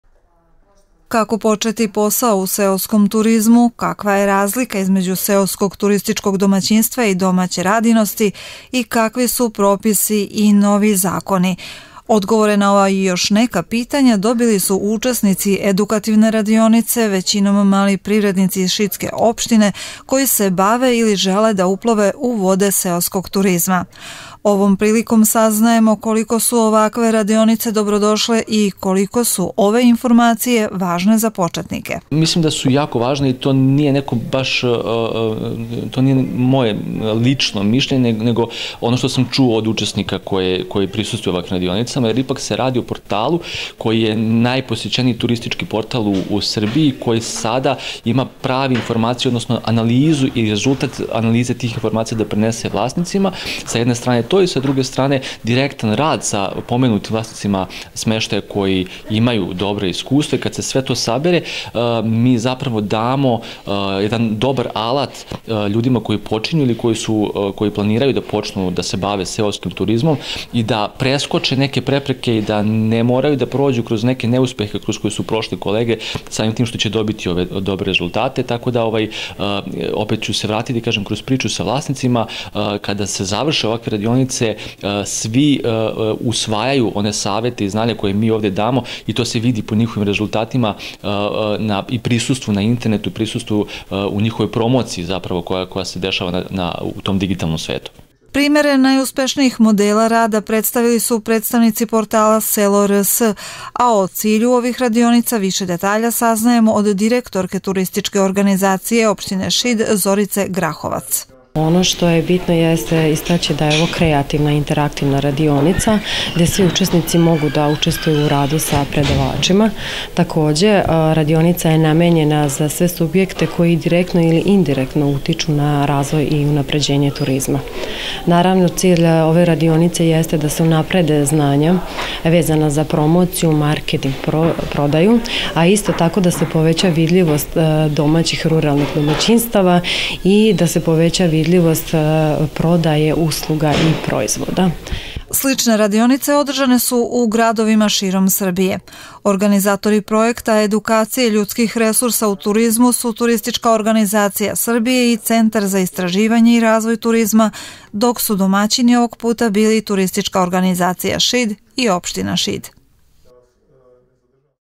Edukativna radionica
sali skupštine opštine Šid danas je održana radionica za edukaciju ljudskih resursa u turizmu na kojoj su učestvovali maliprivrednici iz šidske opštine. Domaćini su bili Opština Šid i Turistička organizacija opštiine Šid